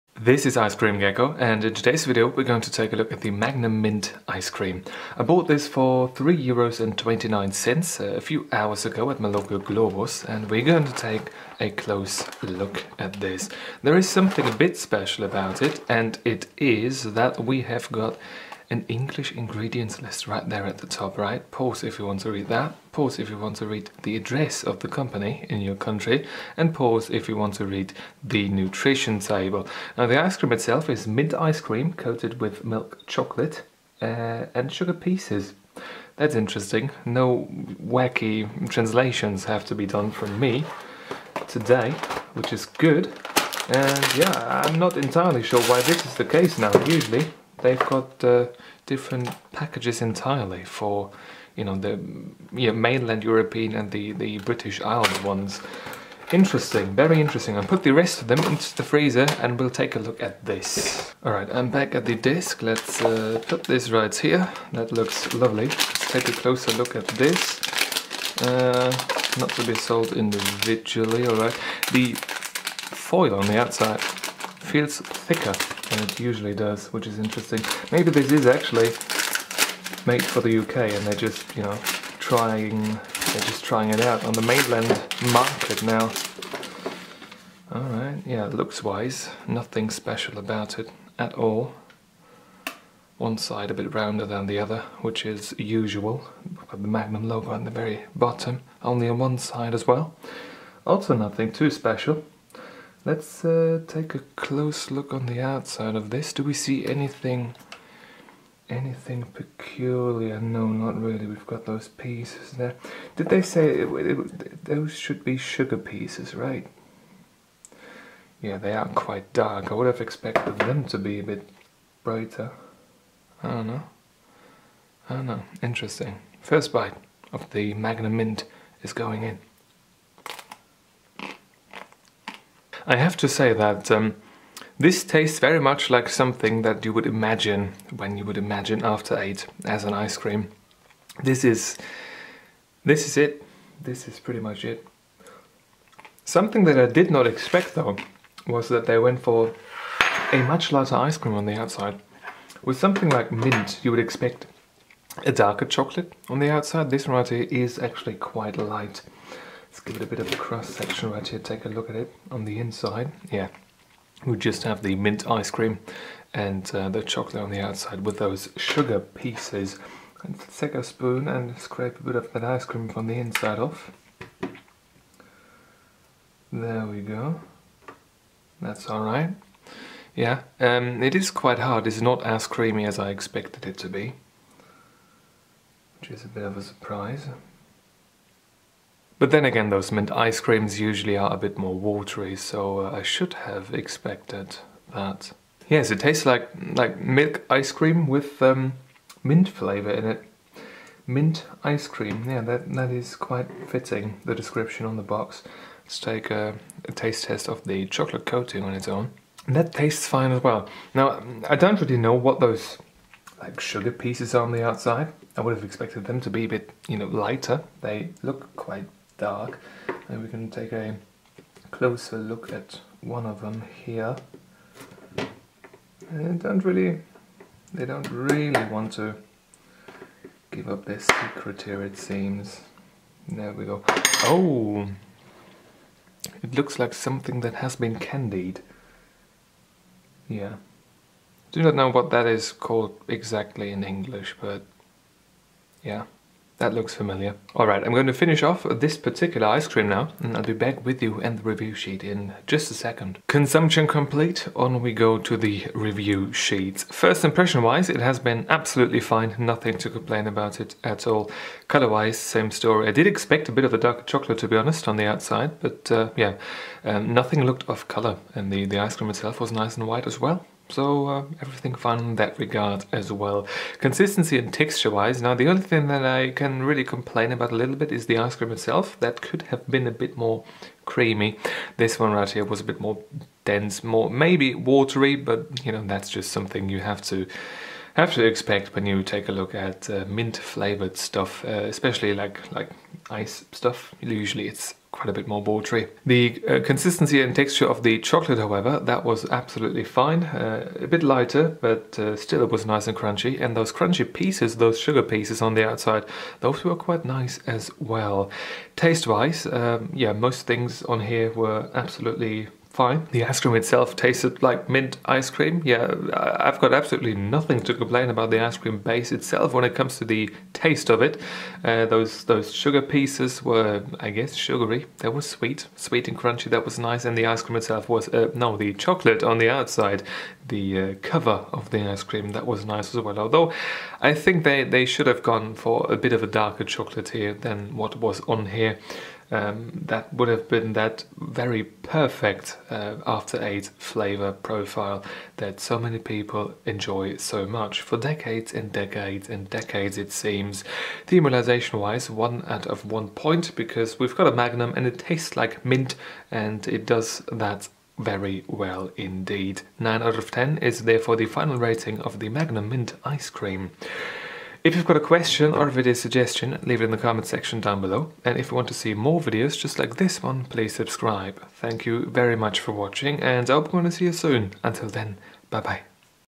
Magnum Mint Review
Today we are taking a look at some ice cream that is completely new to me. I do not know if this was ever on offer over here but I would expect this to have the grunt to become a classic indeed. After-Eight as an ice cream is quite pleasant overall.